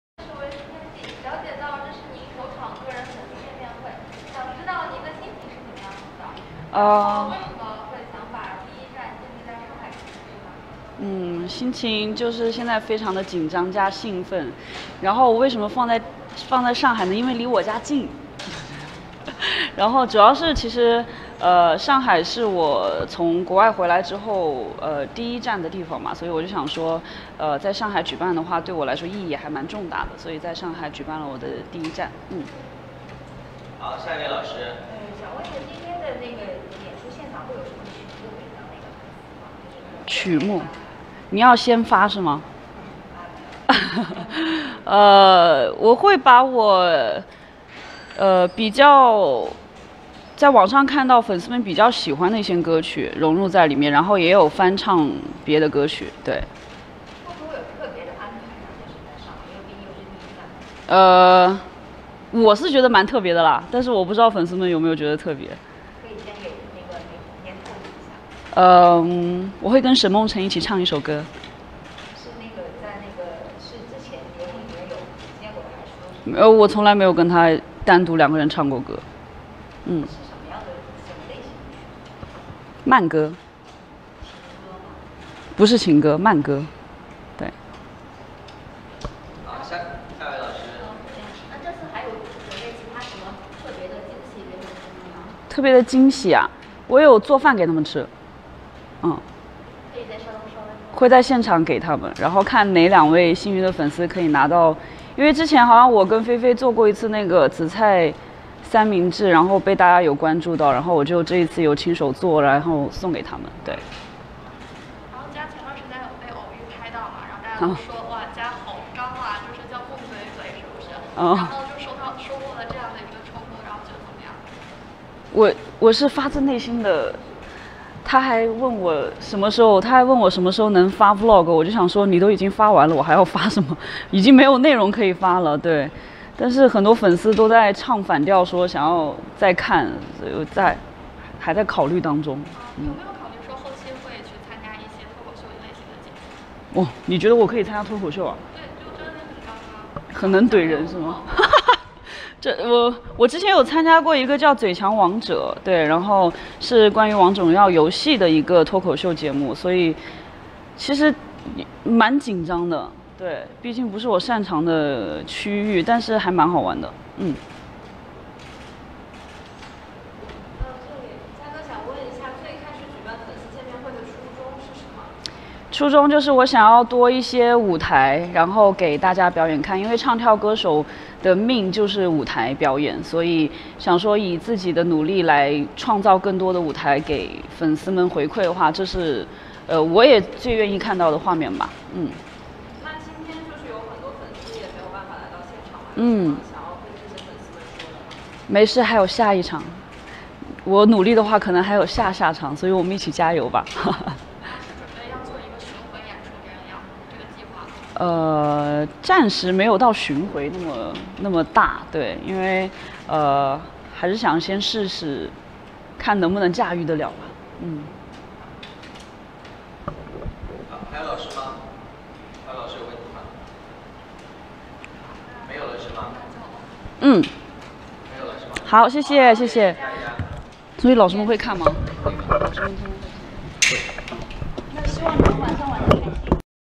孟佳首场个人粉丝见面会媒体采访 Meng Jia 1st Solo Fan Meeting Media Interview 20210626
孟佳首场个人粉丝见面会《首站|上海》
Meng Jia 1st Solo Fan Meeting 《1st Station | Shanghai》

Cr:佳霏猫应援站微博 20210626